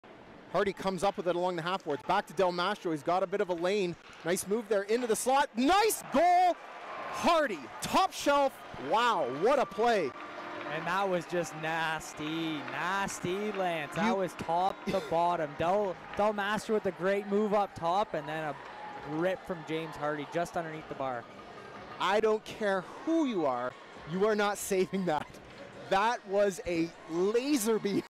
OHL Play Of The Night | Ethan Del Mastro | December 29, 2021
OHL Play of the Night: How 'bout That Pass From Ethan Del Mastro?!

Ethan Del Mastro feeds James Hardie to earn play of the night honours.